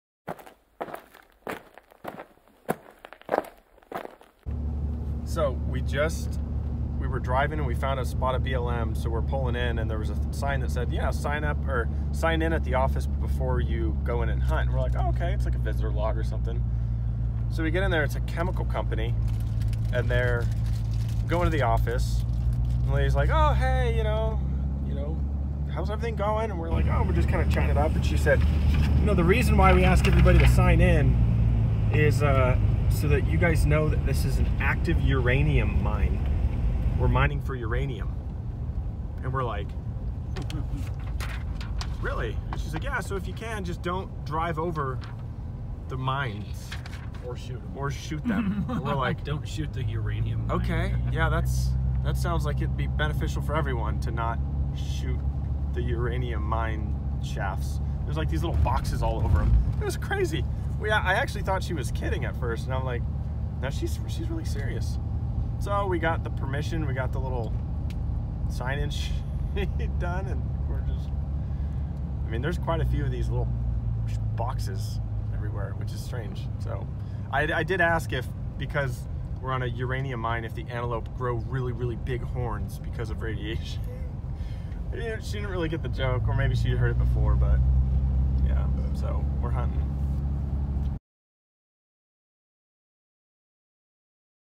We hunted in an active URANIUM MINE!
We went out to Wyoming abs got access to antelope hunt in an active uranium mine! Here is the video we got during the hunt!